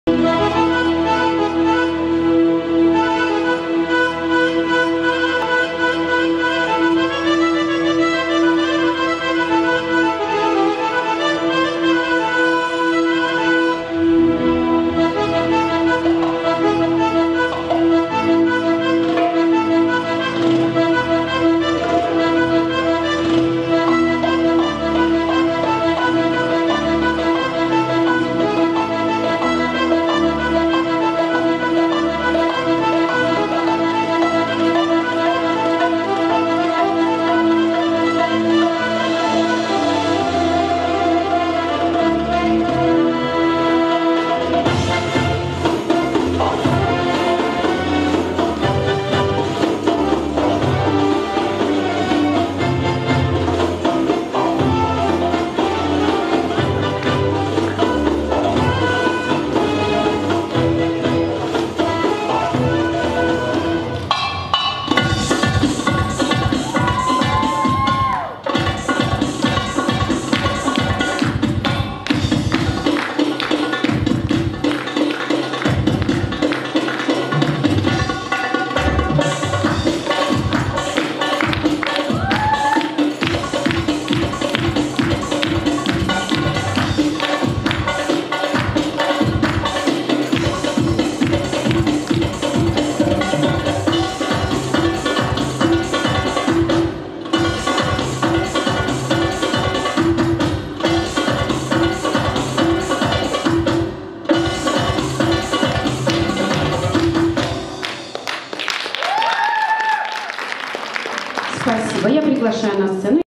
Марина Дудинская, Импровизация / "Ассорти 2017" Юбилейный фестиваль.
Юбилейный фестиваль "Ассорти 2017" (Минск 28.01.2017) //
Школа танцев "Вверх". Тренер - Сенчихина Евгения